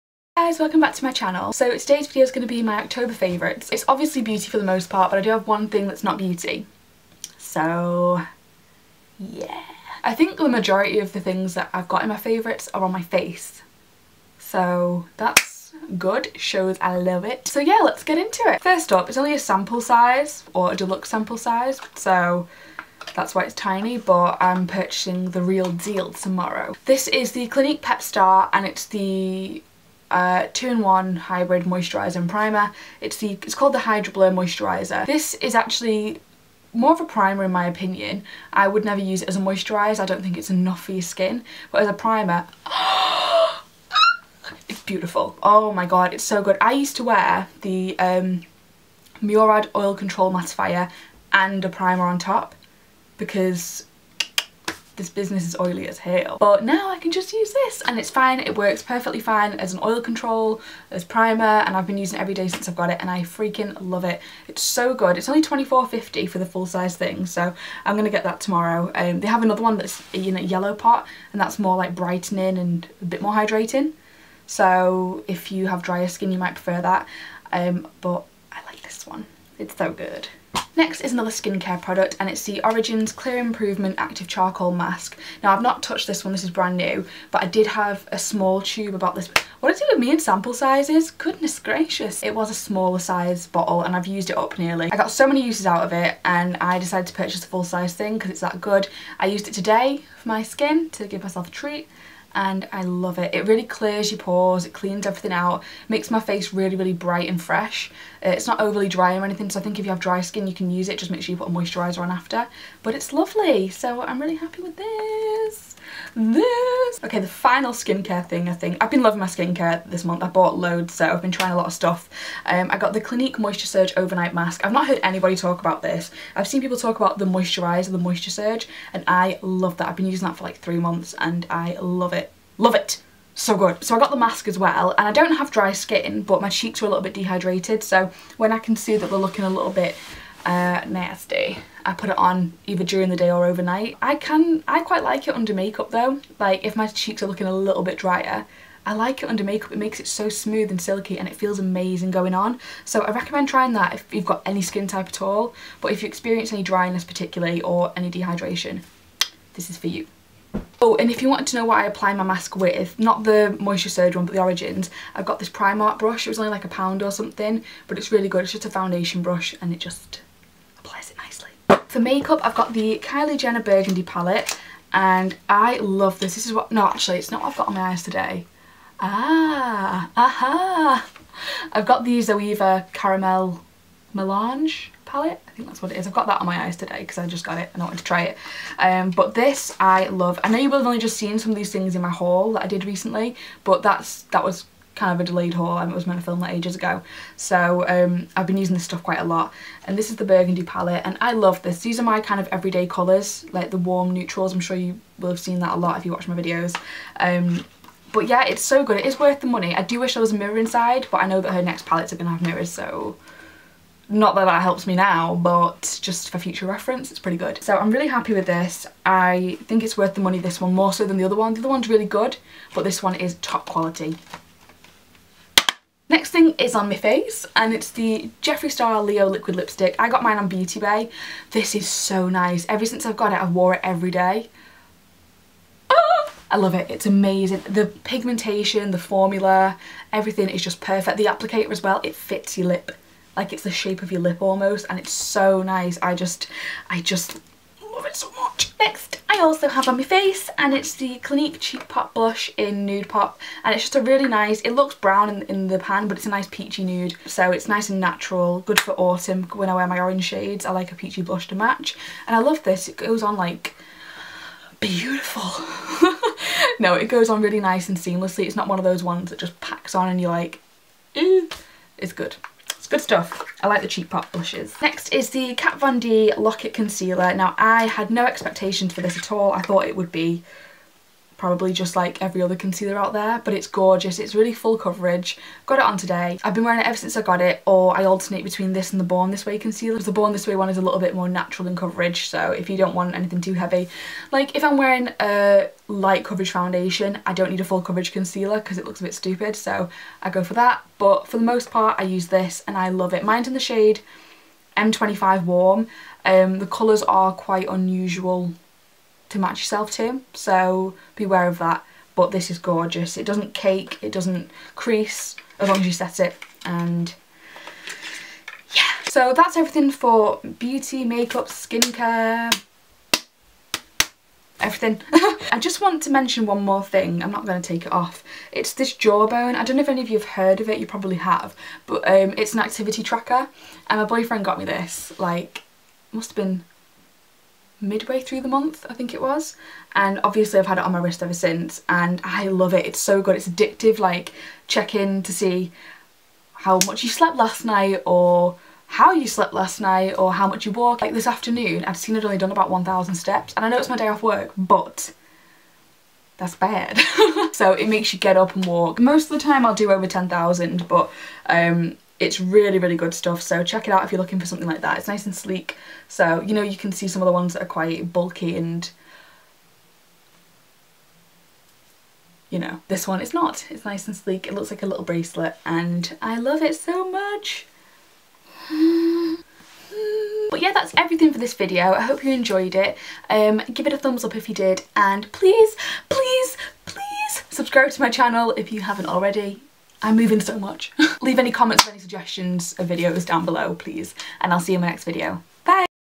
October Favourites| Nicole Courchée
Products Mentioned
Clinique Hydroblur Pep Start
Origins Charcoal Mask
Clinique Moisture Surge Mask
Primark Rose Gold Foundation Brush
Jeffree Star Liquid Lipstick in Leo
Clinique Cheek Pop in Nude Pop
Kylie Cosmetics Burgundy Palette
Kat Von D Lock It Concealer in M25 Warm
Jawbone Activity Tracker